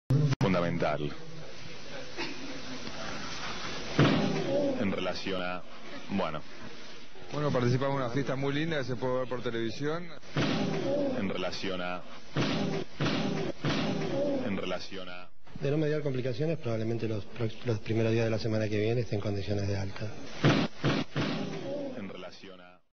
Marci se golpea con una pizarra xD
Mauricio Marci estaba por dar conferencia de presa y cuando llevan una pizarra, lo golpea en la cabeza.